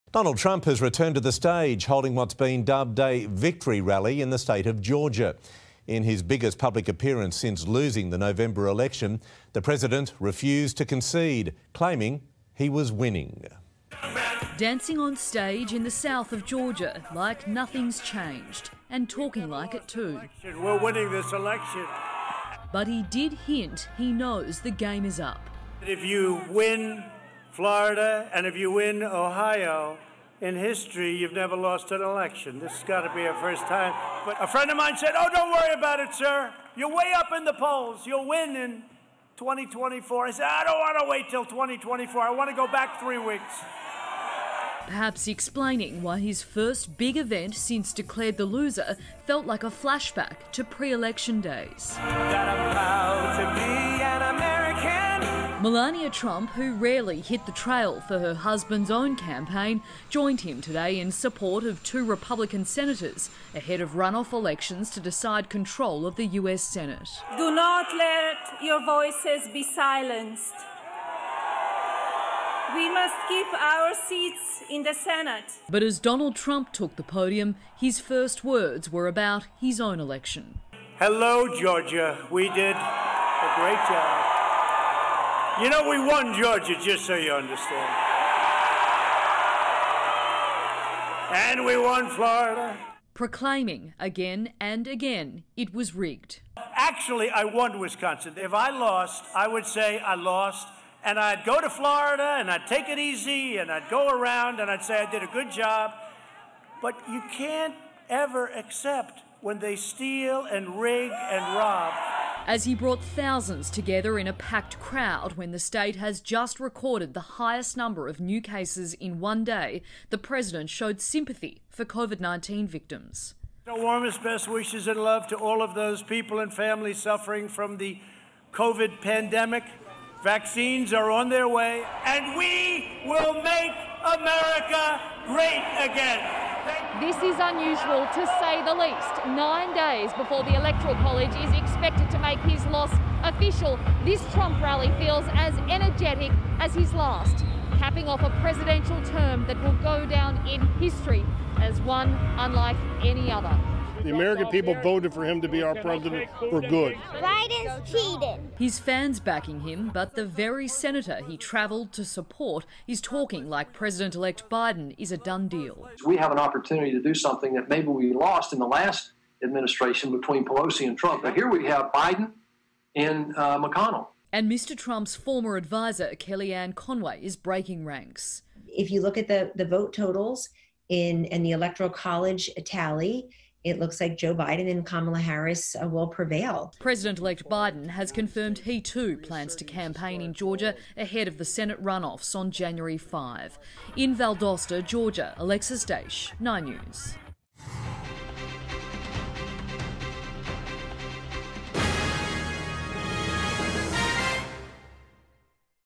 Trump refuses to concede in Georgia ‘victory rally’ | 9 News Australia
US President Donald Trump has refused to concede, claiming he was winning in his biggest public appearance since losing the election in Georgia.